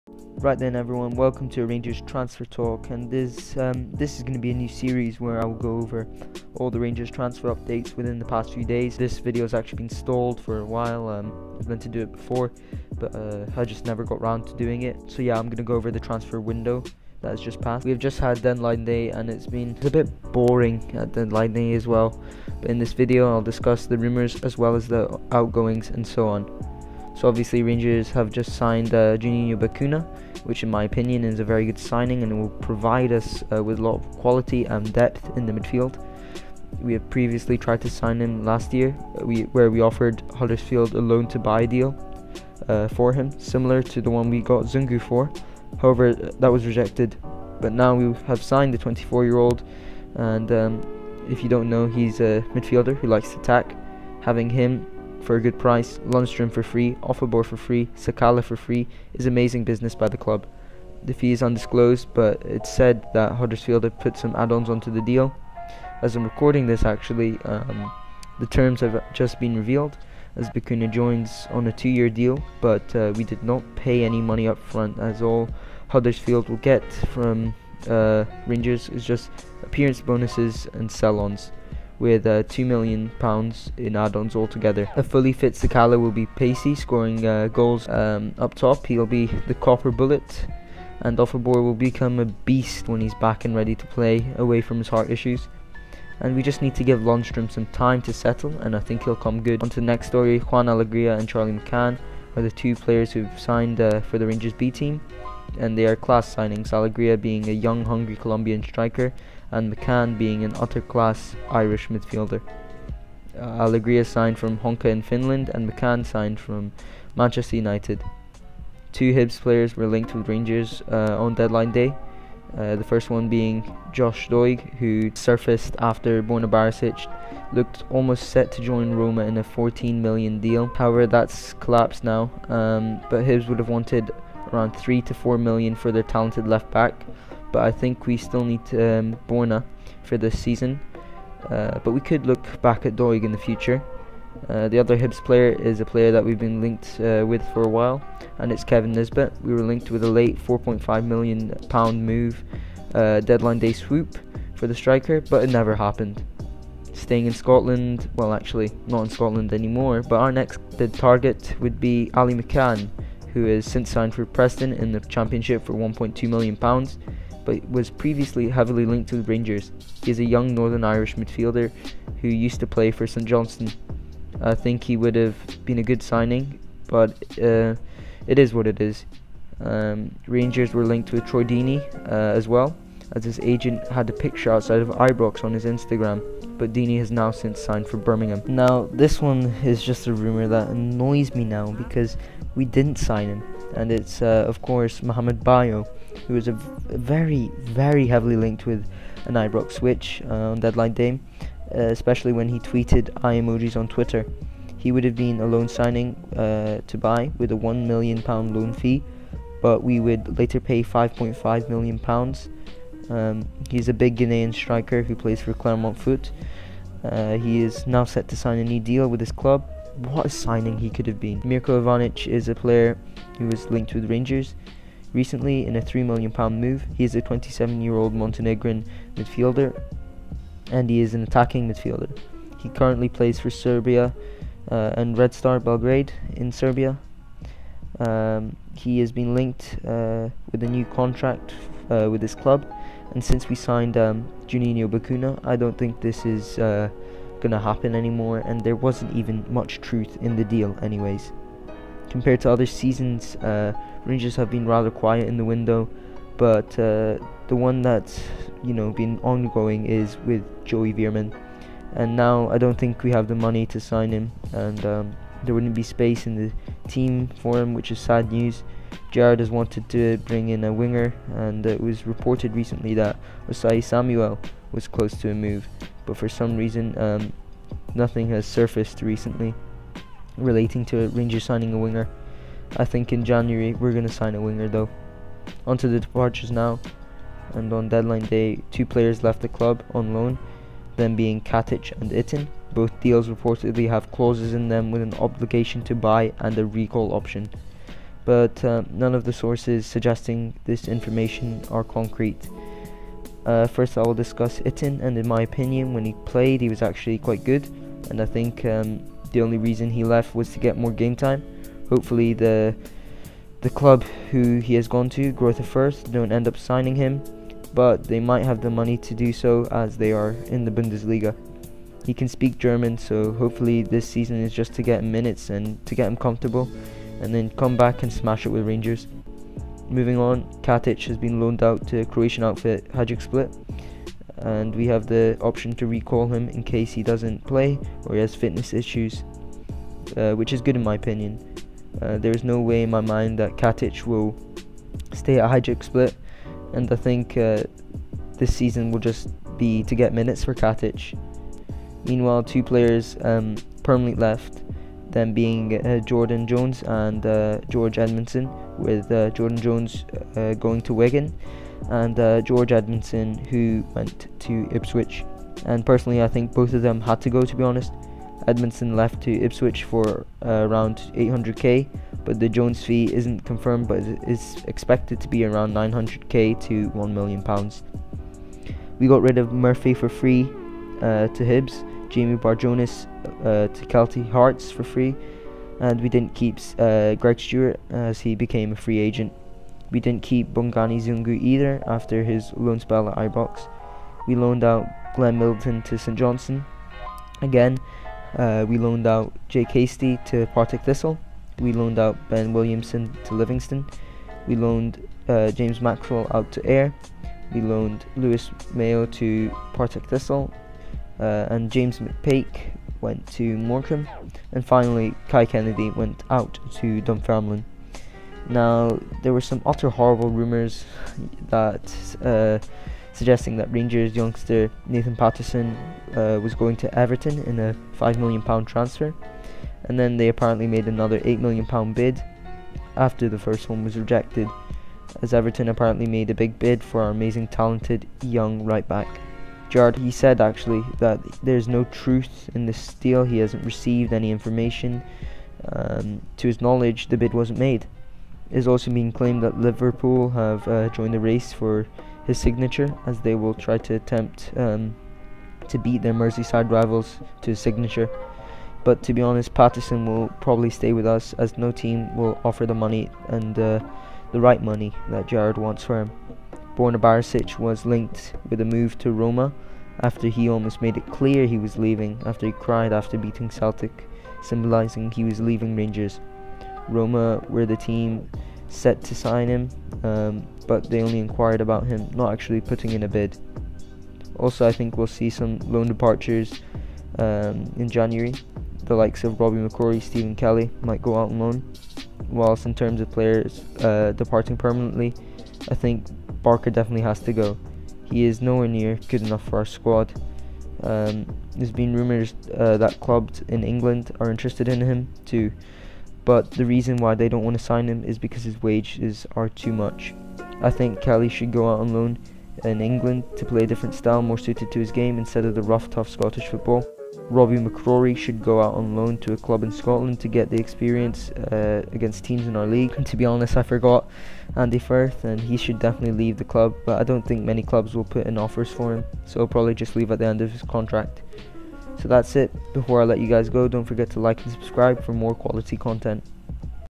RANGERS TRANSFER TALK - THE TRANSFER WINDOW
Today I go over the players that Rangers have signed and the players that we have sold in the transfer window.

Rangers Dubai